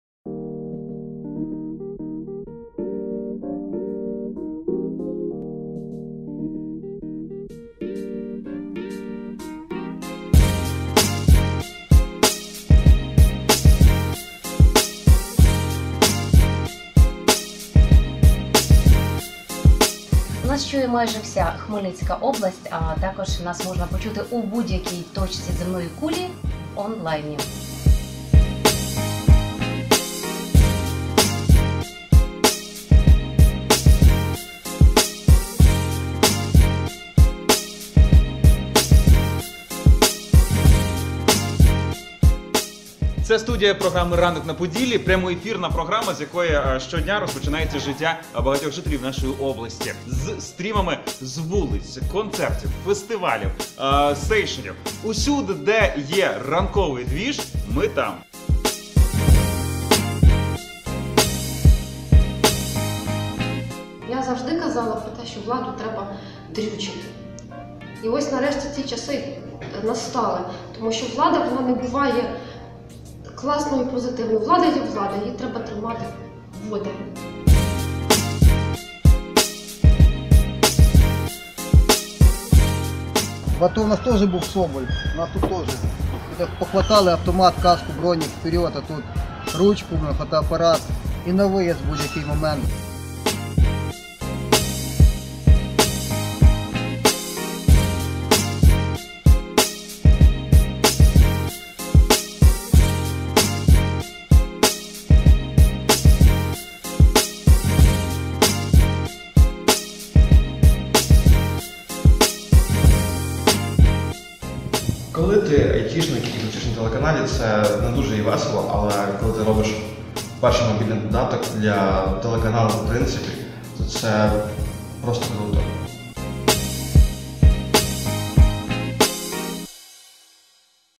Мобільний фільм про телеканал
В рамках фестивалю мобільних фільмів було виготовлено мобільний фільм про наш телеканал за 1 добу, приурочений до дня журналістики.